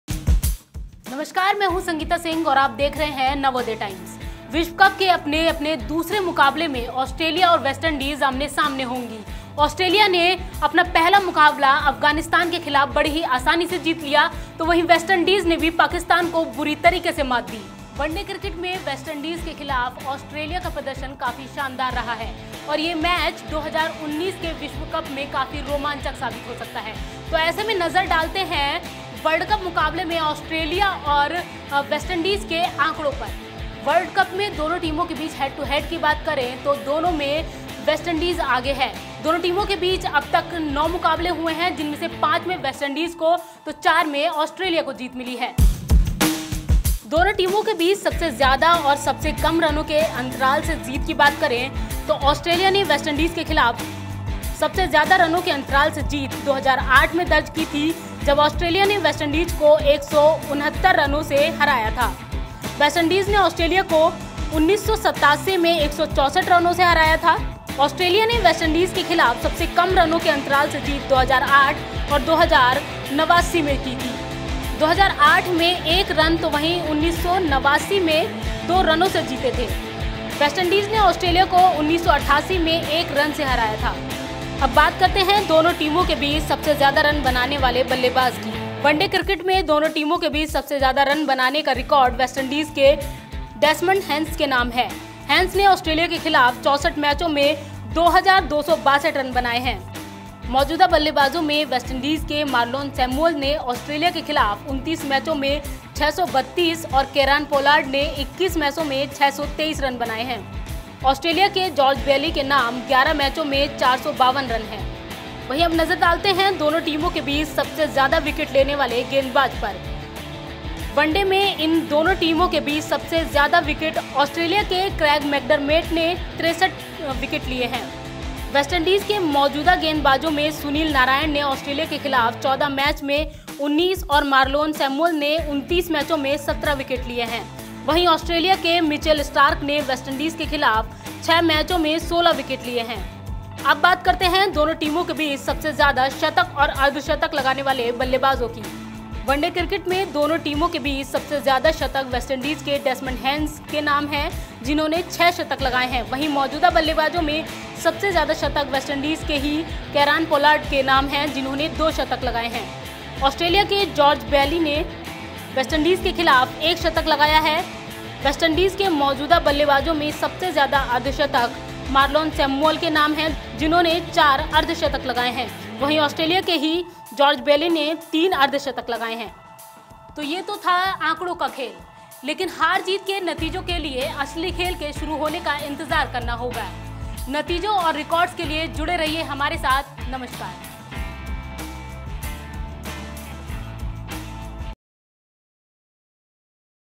रोमांचक होगा Australia Vs West Indies मुकाबला, जानिए आंकड़ो में कौन है आगे
रोमांचक होगा Australia Vs West Indies मुकाबला, जानिए आंकड़ो में कौन है आगे. विश्व कप के अपने-अपने दूसरे मुकाबले में ऑस्ट्रेलिया और वेस्टइंडीज आमने-सामने होंगी। ऑस्ट्रेलिया ने अफगानिस्तान के खिलाफ पहला मुकाबला आसानी से जीता था। वेस्टइंडीज ने भी अपने पहले मुकाबले में पाकिस्तान को बुरी तरह से हराया था। वनडे क्रिकेट में वेस्टइंडीज के सामने ऑस्ट्रेलिया का प्रदर्शन शानदार रहा है और यह मैच 2019 विश्व कप का सबसे रोमांचक मैच हो सकता है। तो आइए नजर डालते हैं..वेस्टइंडीज और ऑस्ट्रेलिया के विश्व कप और वनडे मुकाबले के आंकड़े पर।